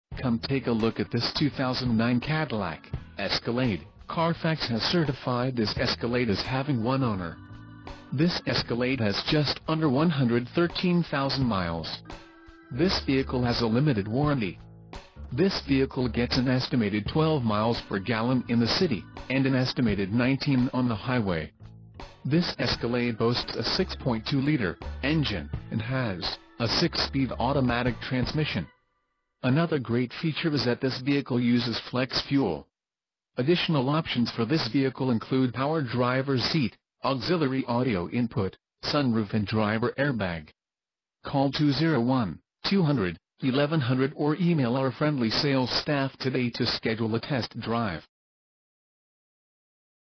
2009 Cadillac Escalade AWD 4dr SUV - Jersey City, NJ
New Jersey State Auto Auction, Jersey City, NJ - Cadillac Escalade AWD Leather Sunroof Backup Camera T.V DVD 1-Owner